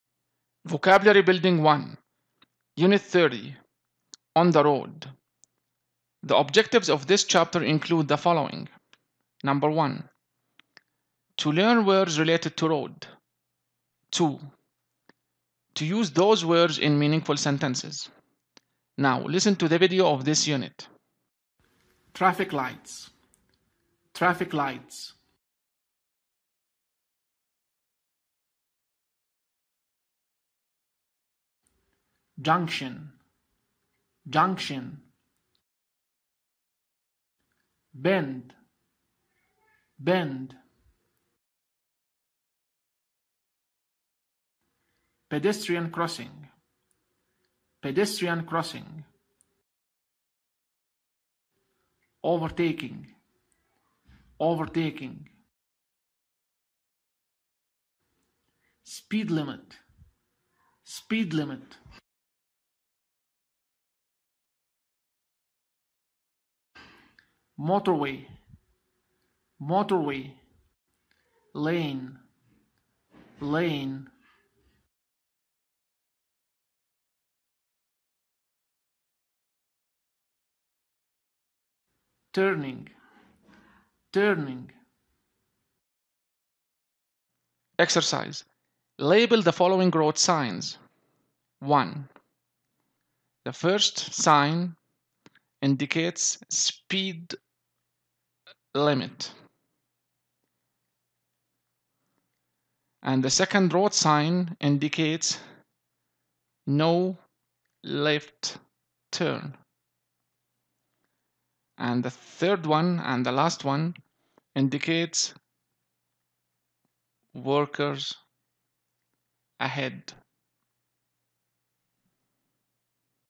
Vocabulary Building (1) | U - 30 | On the Road
المقررات المفتوحة - مقرر  Vocabulary Building - 1
الوحدة 30 : On the Road
• To learn words related to road.
• To use those words in meaningful sentences.